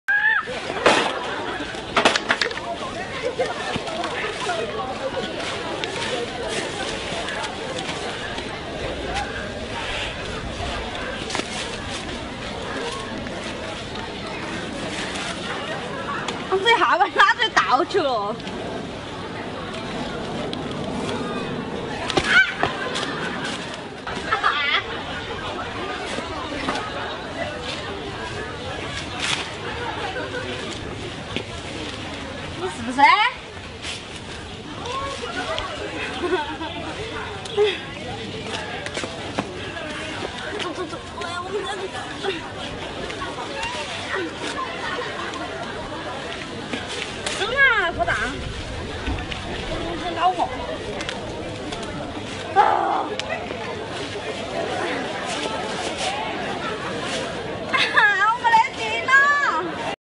Baby pandas making mess funny
These cute little panda bears can make a big mess so you nedd watch them all the time. Cute panda bears making mess in the zoo. Can't stop cute little pandas from making mess they like it. Try not to laugh while watching cute little pandas making mess.